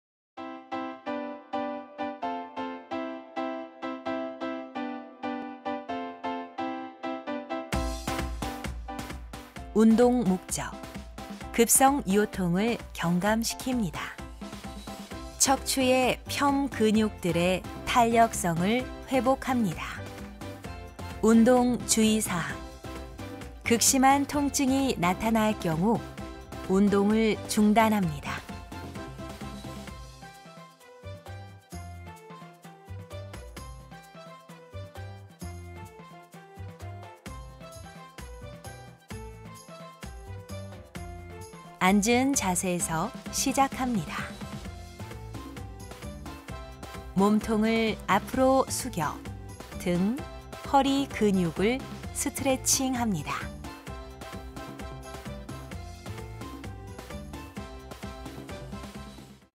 98 맥킨지허리운동 6
부산대학교병원 근골격계 운동법 영상입니다
올바른 자세의 스트레칭으로도 통증을 완화시킬 수 있다는 사실!
모두 함께 따라해볼까요?